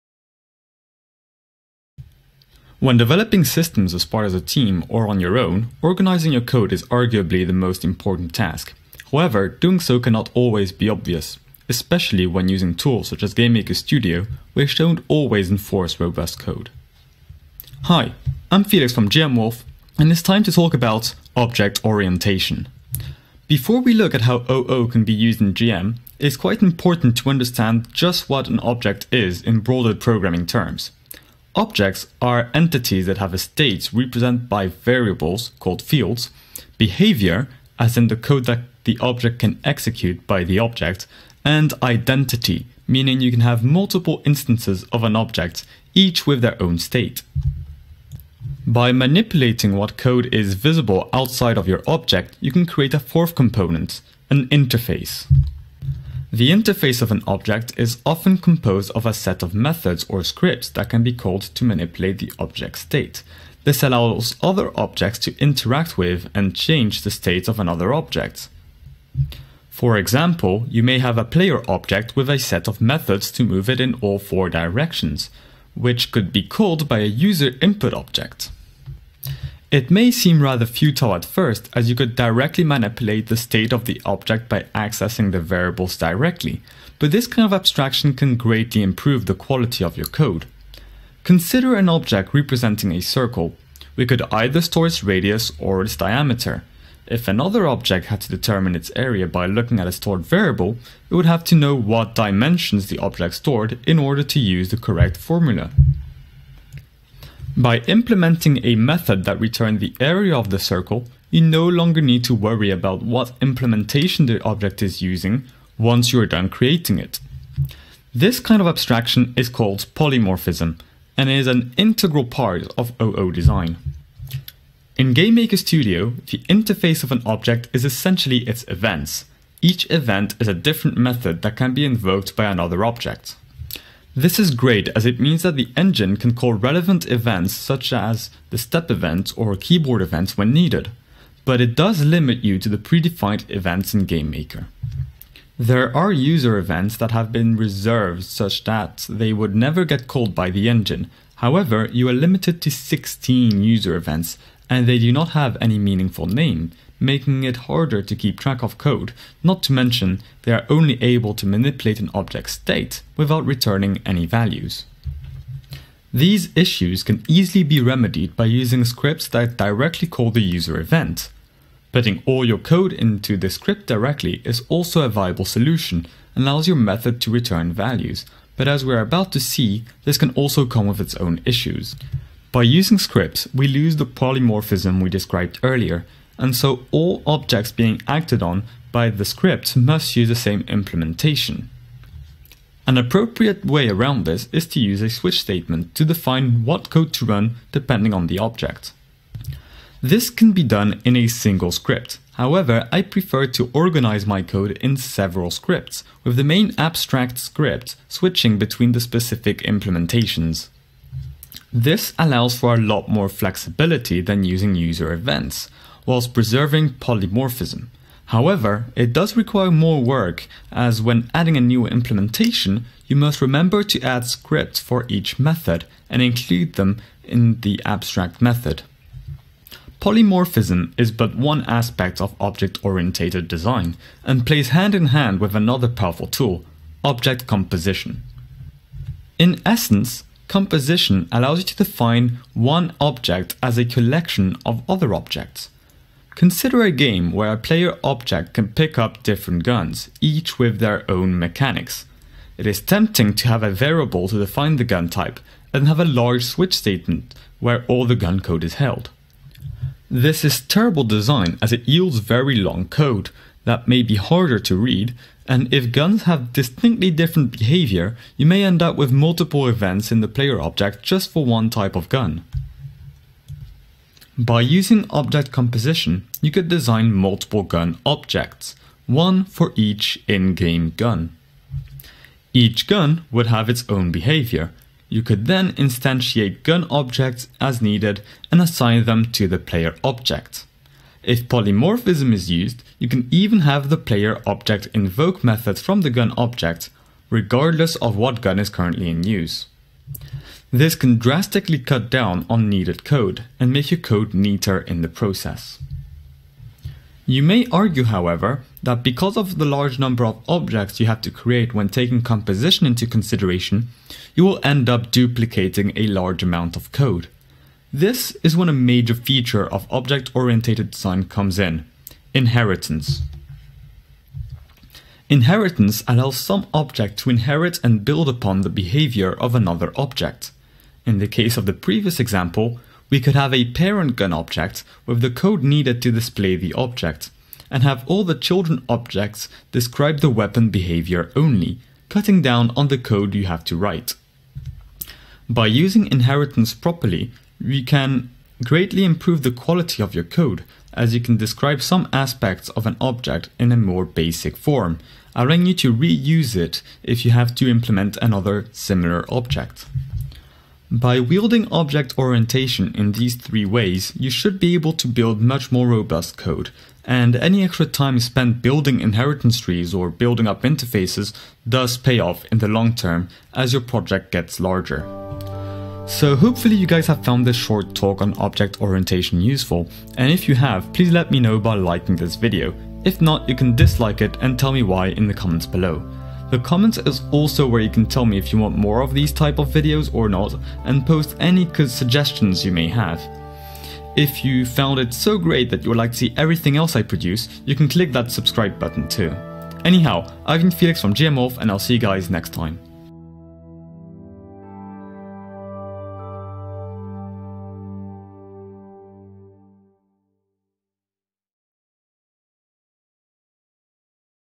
Object Orientation - GMWolf
Today i will be talking about object orientated design, the challenges that GM may pose, and how to overcome them.